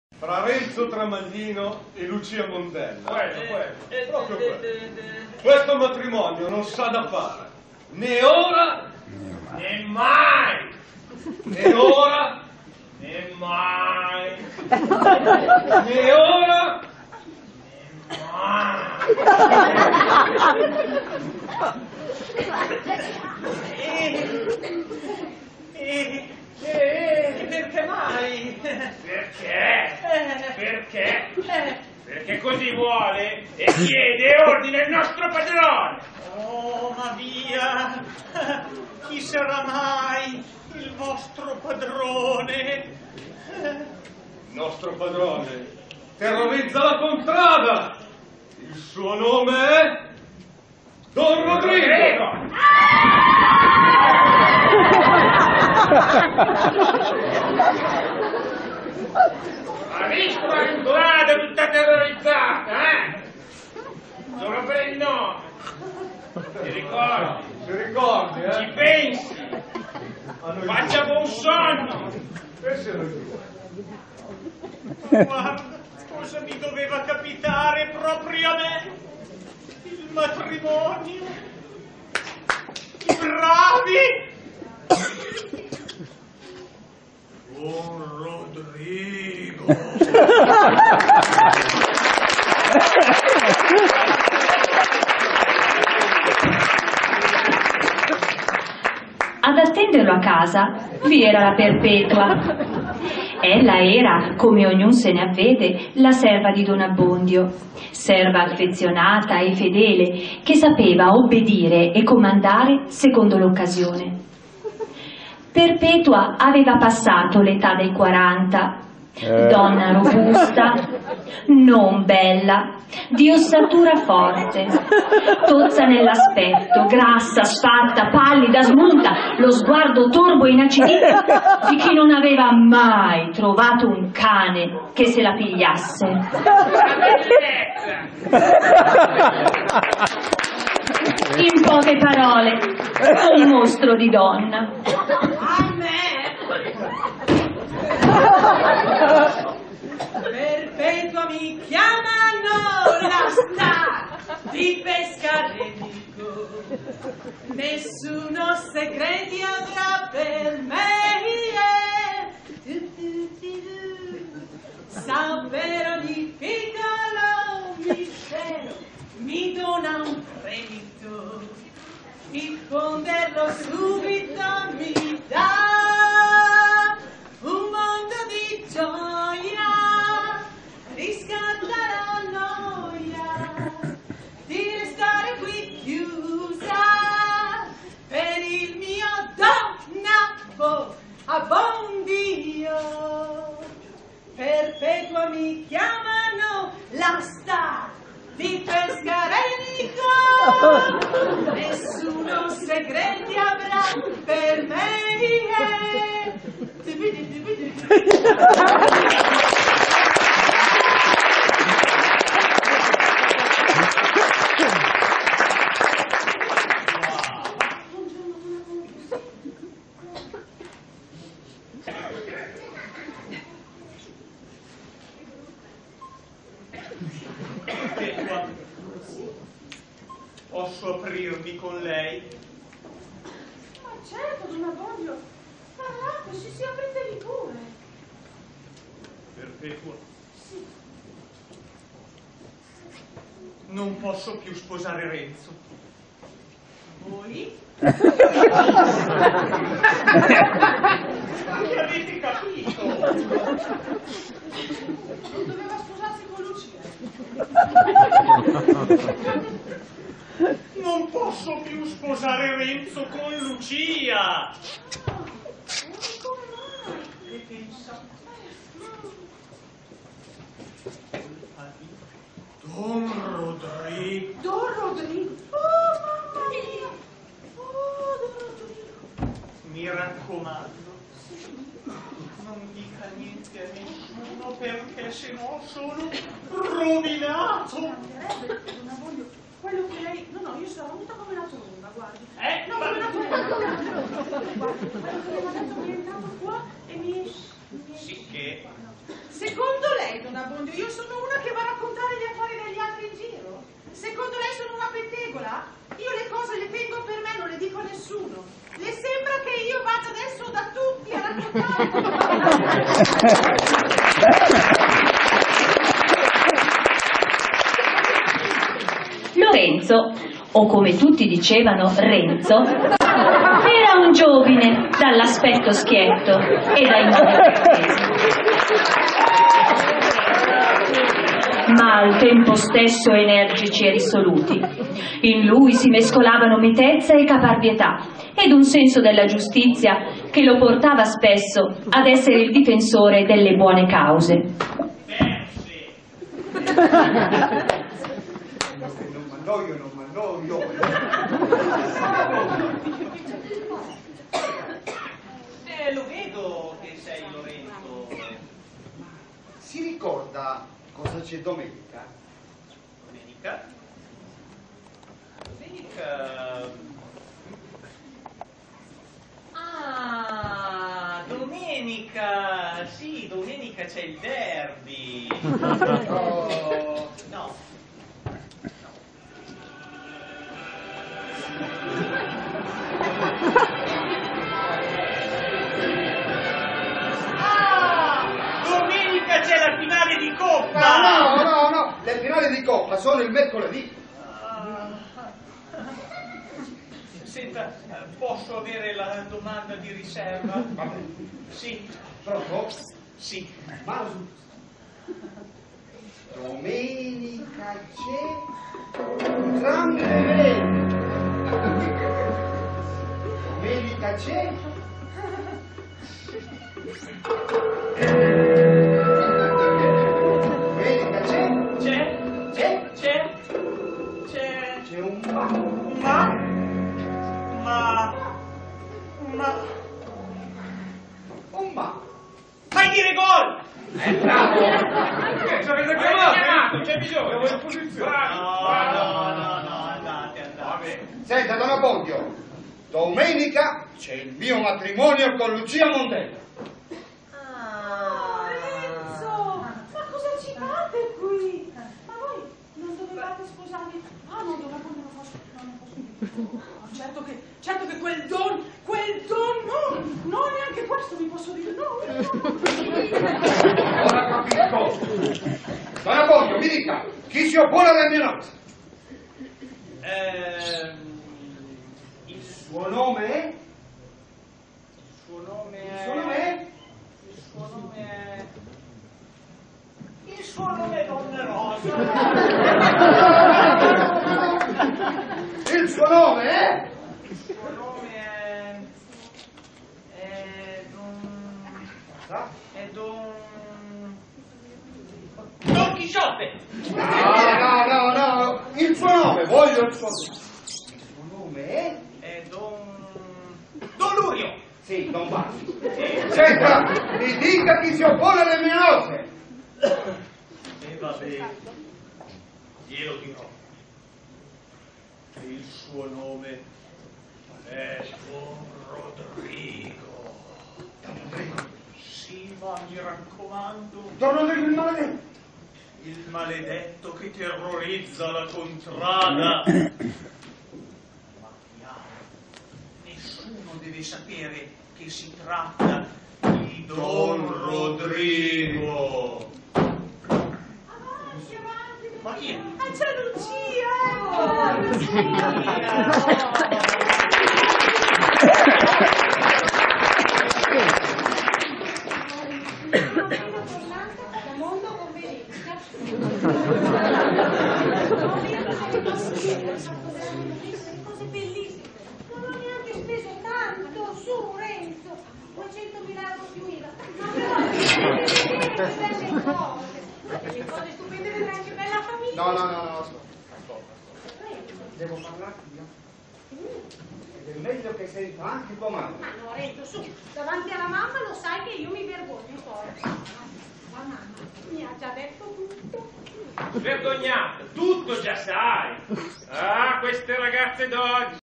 san_rocco_recita_natale09_(006).mp4
Recita di Natale dei ragazzi, giovani e genitori dell' oratorio San Rocco in Voghera 19/12/09